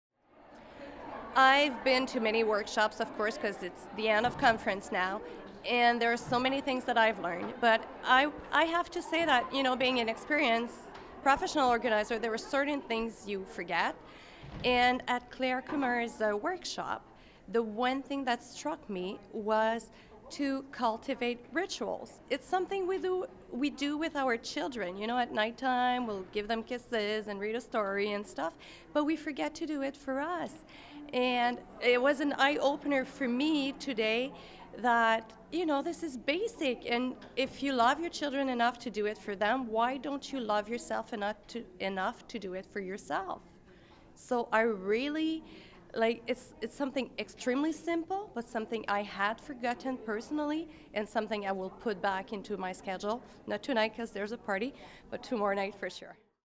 Clare Kumar - Speaker Praise from Natalie Bureau on the concept of "Rituals"
Professional organizer, Natalie Bureau shares the most powerful point she took away from Clare's presentation on Personal Productivity.